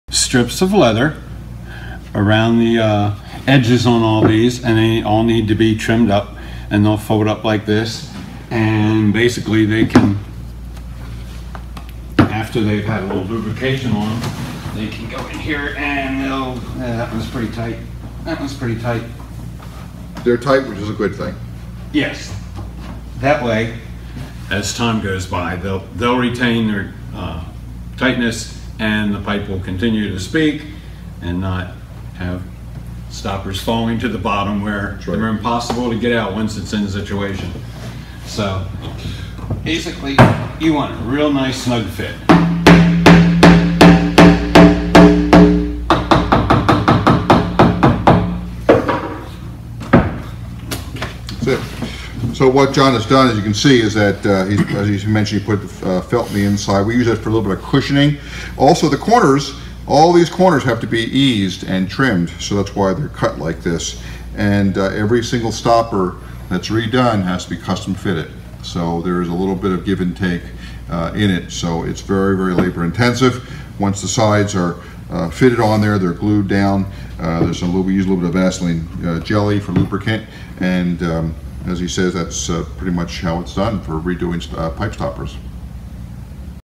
Patrick J. Murphy Pipe Organ Stoppers
While these featured pipes are not part of the St. Francis Cathedral Pipe Organ, this was the type of restoration work done on similar pipes.